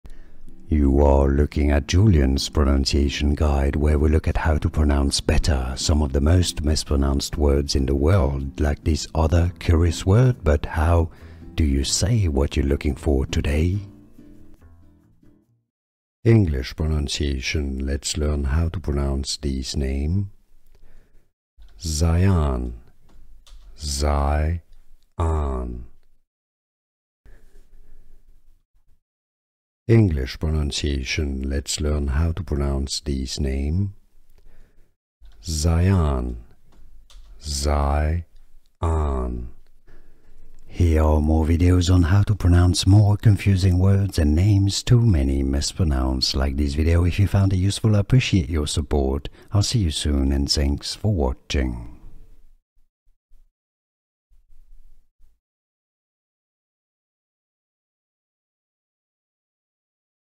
How to Pronounce Zyan (Correctly!)
Welcome! Here you will hear How to Pronounce Zyan (Correctly!) i.e. the “correct” pronunciation.

You can skip the intro through the time stamps below:
00:00 - Pronunciation Intro
00:15 - How to Pronounce Zyan (Correctly!)